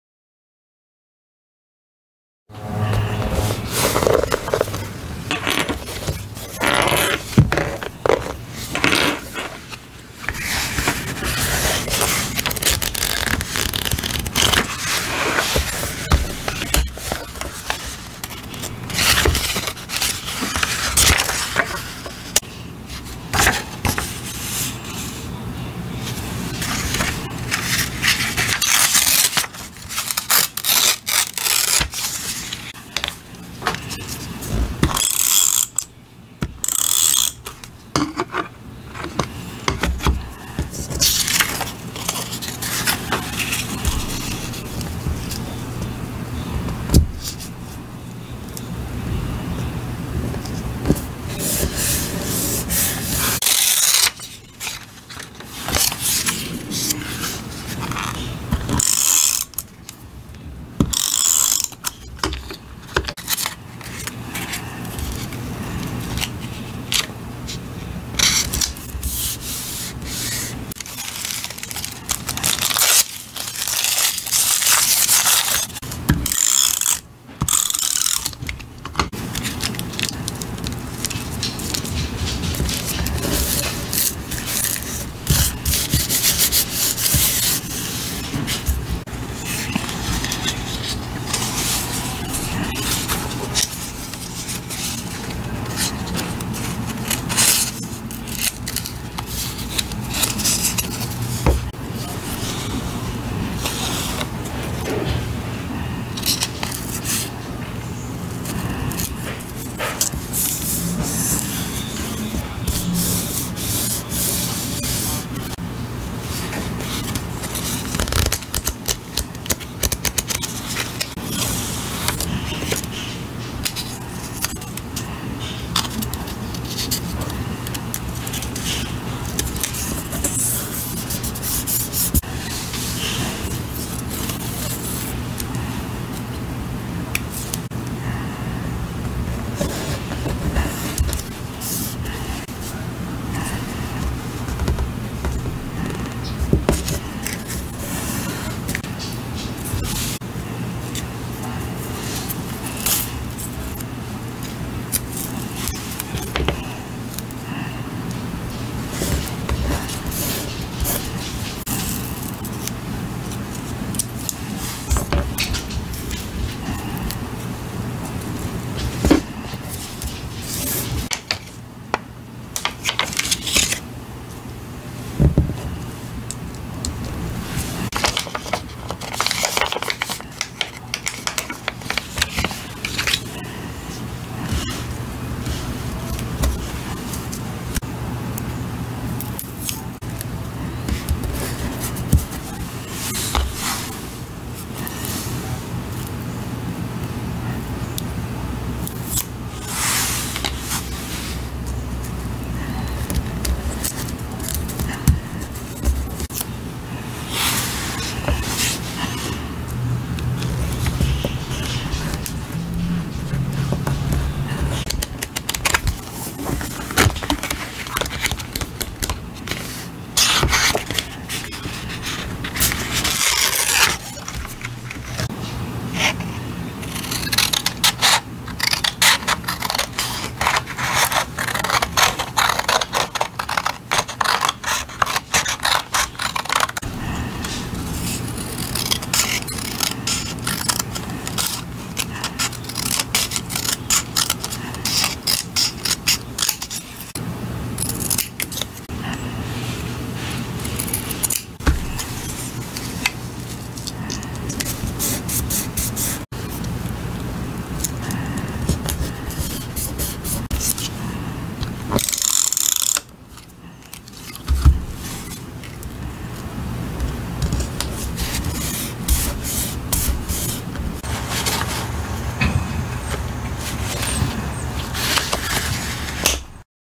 Scrapbooking/Journaling |ASMR
I had so much fun making this video!
It was my first attempt at scrapbooking, sorry if it's not the best! I'm hoping to make more of these videos because they're so fun to make :).